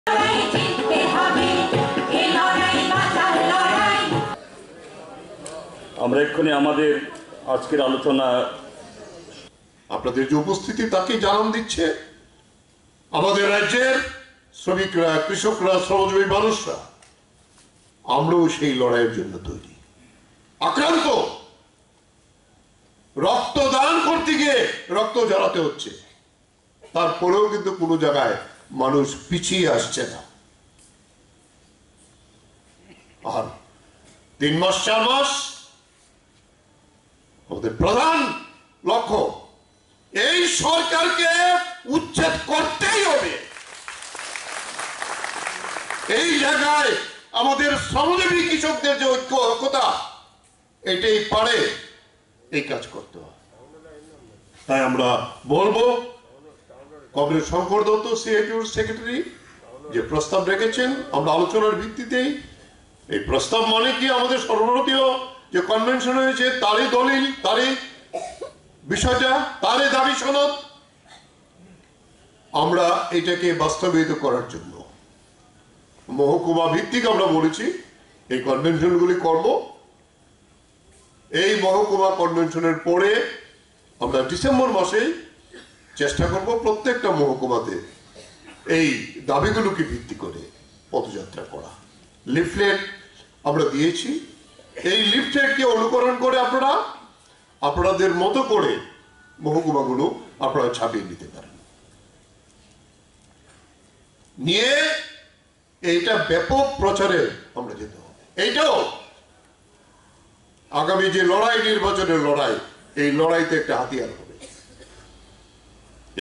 Pabitra Kar Speaks at State Conference of AIKS on Nov.13,'22#agtlive24x7#
agtlive24x7 Augments the Truth